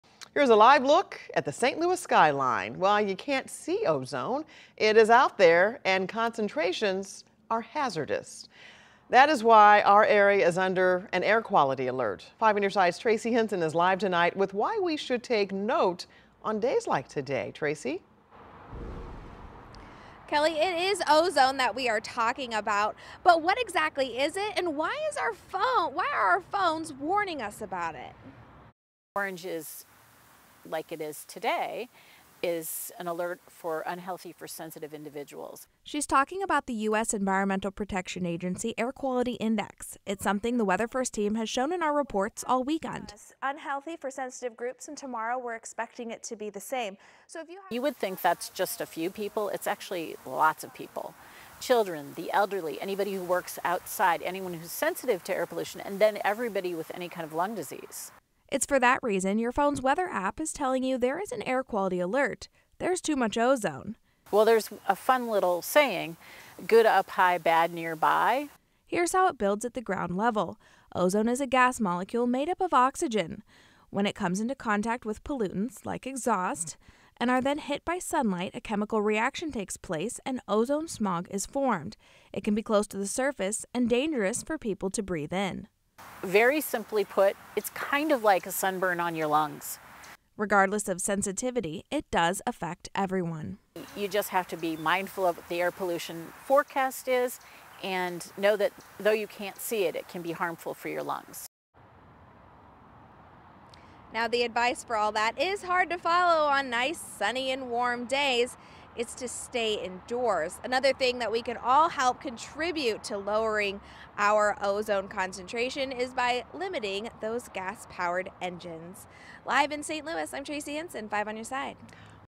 Explainer: What's an ozone? Why are air quality alerts issued for high concentrations?
Here's what ozone is and why air quality alerts are issued for high concentrations. The St. Louis-area has been under an "Orange - Unhealthy for Sensitive Groups."